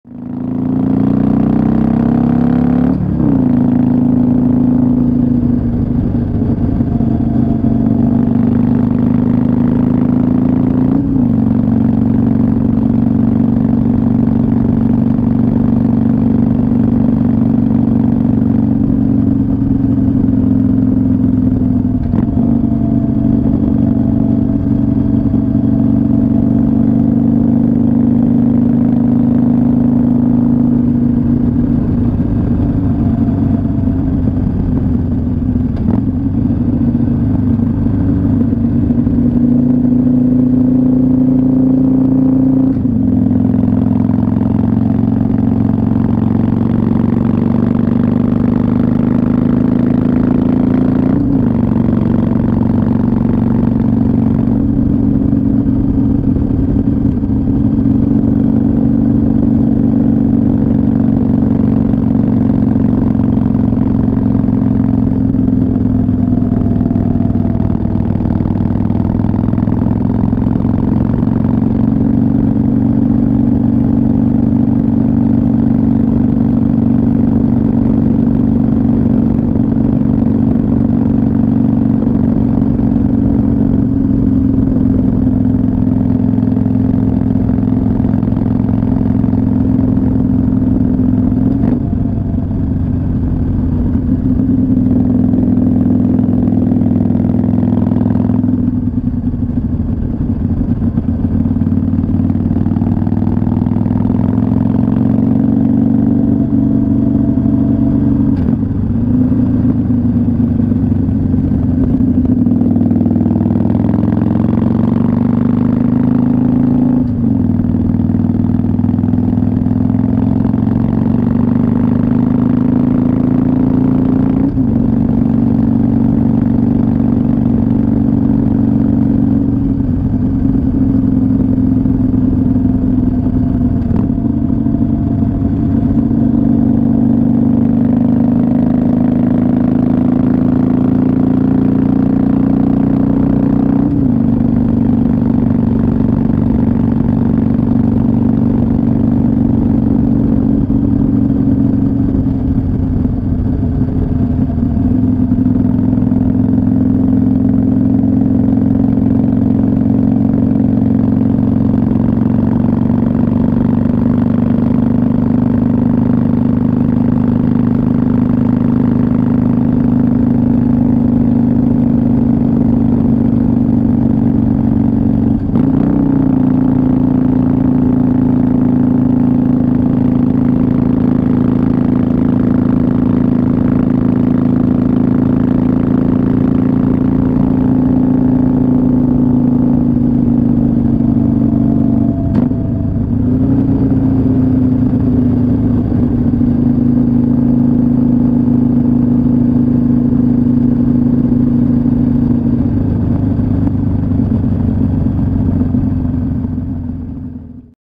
Epic Harley Davidson Exhaust Audio! Growling Iron 883 with Vance & Hines Pipes!
Turn up the volume! No chat, no music, just three and a half minutes of pure Harley exhaust audio awesomeness.

Testing my new Chin Mounts GoPro mount (and new helmet) with a cruise along some country roads on a sunny afternoon. You just can't beat that Harley Davidson feeling! It'd really make my day. 🙂